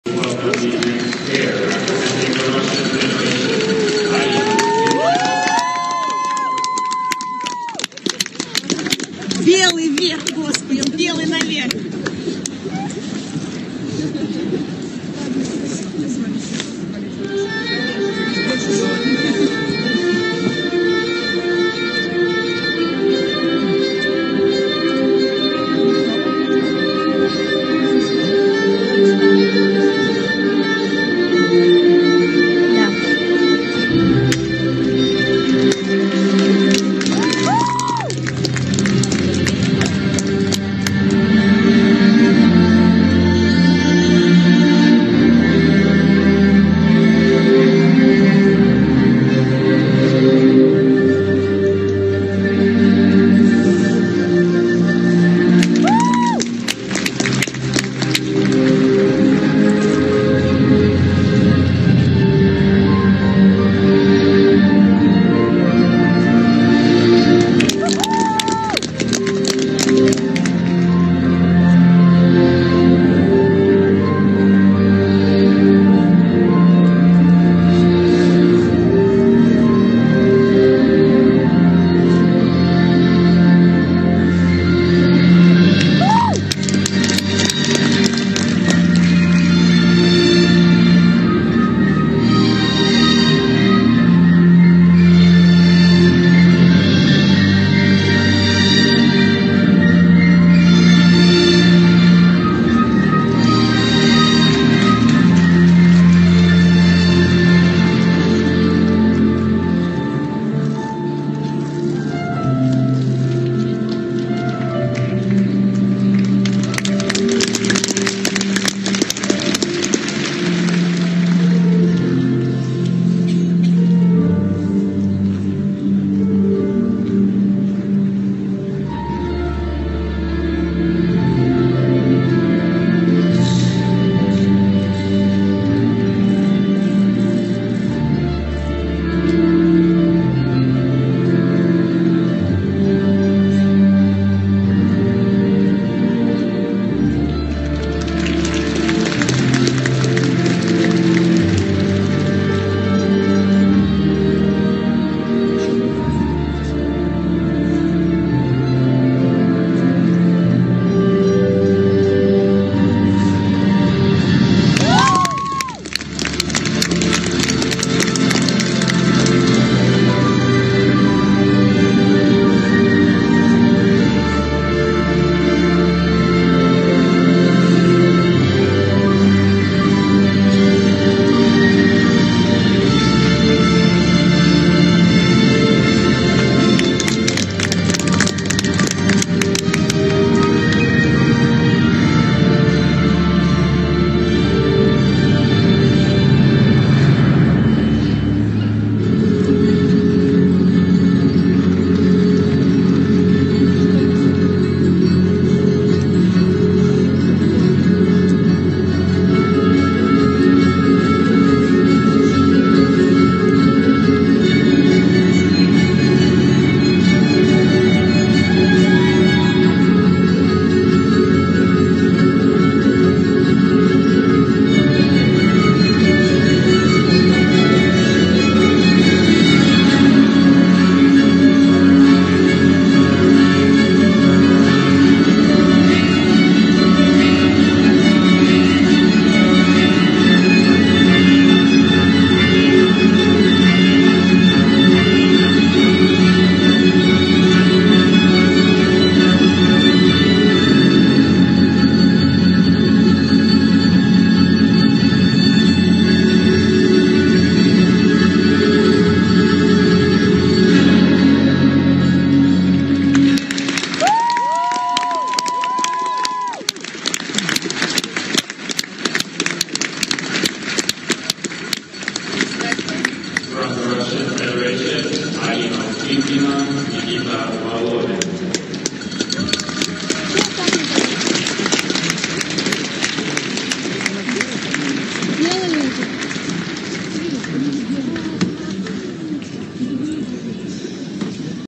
USTIMKINA Alina / VOLODIN Nikita (RUS), free programm, LP, FP, YOG 2016
Алина Устимкина - Никита Володин (Россия), произвольная программа на Юношеских Олимпийских играх. Запись ПП с трансляции из Перископа Анастасии Шпилевой. Спасибо!